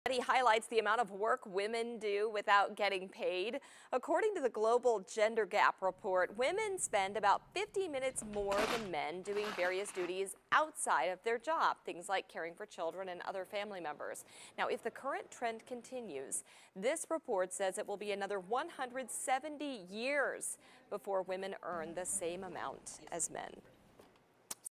Women work more than men according to study
According to the new Global Gender Gap Report, women work more than men on a daily basis.